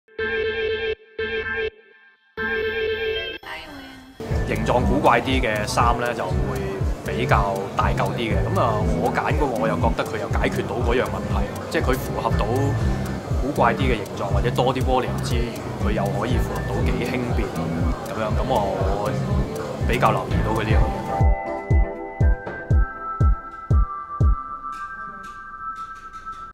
izzue x Tsinghua Fashion Design Award 2014 - HK Semi Final - Honorable Judge, Kiu Leung
The stellar panel including fashion designers, stylists, editors and influencers has joined us in selecting the finalist to represent Hong Kong in the global final at Tsinghua University, Beijing. Here are some inspiring comments by Kiu Leung,Deputy Editor-in-Chief of
EASTTOUCH   .

是次比賽的專業評審團陣容強大，由時裝設計師、造型師、時尚名人及傳媒人組成，較早前已經投票選出他們喜愛的作品，一眾評判在獨家訪問中分享不少具啟發性的觀點、對作品的評語等，一齊觀看東Touch副總編輯 - Kiu Leung的訪問片段，聽聽他對時裝設計的想法和建議。